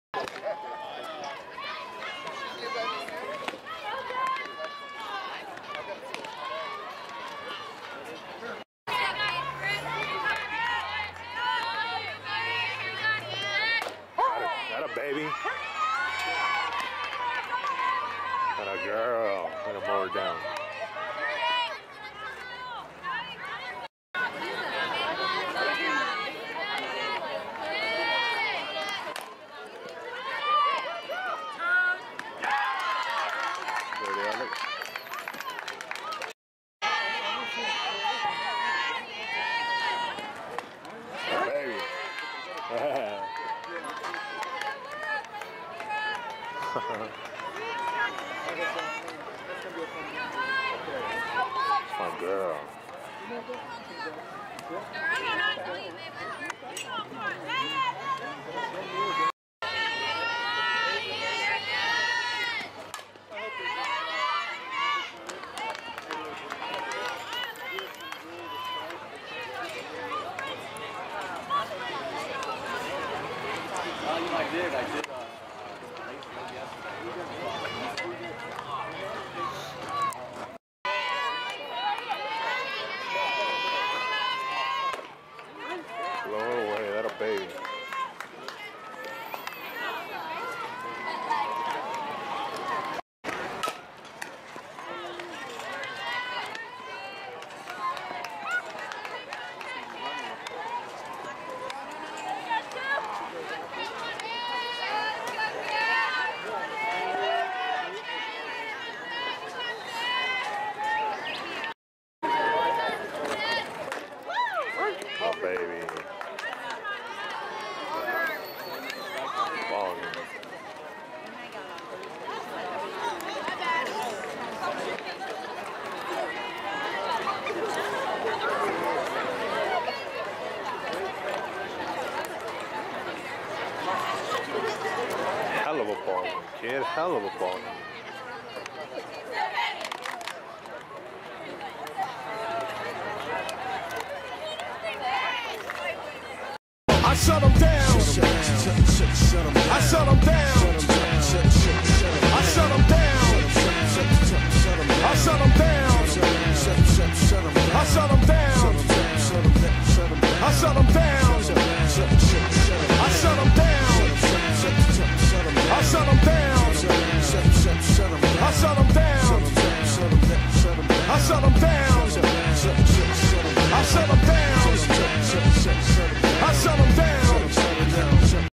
Alex Vargas - Bonita Vista vs Steele Canyon 03/11/14
Bonita Vista defeats Steele Canyon...7-0

Alex tosses a 2-hit shutout in the 1st game of the 2014 season.

Her first game pitching since May 2013!